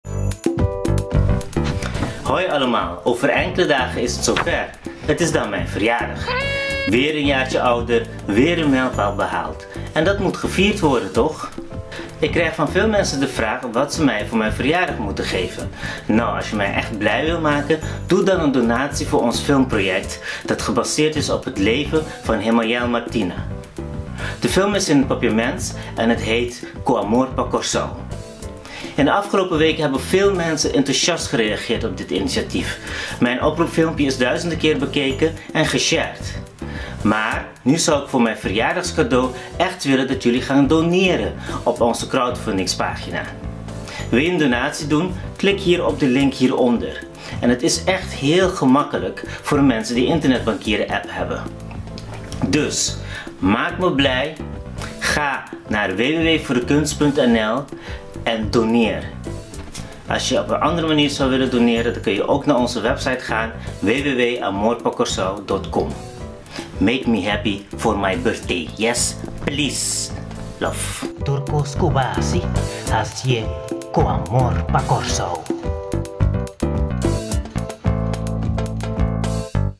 Oproep verjaardag Jermain Lo - Ku Amor Pa Kòrsou
Geef mij het ultieme verjaardagscadeau en doe een donatie voor ons filmproject 'Amor Pa Kòrsou'. We hebben de donaties echt nodig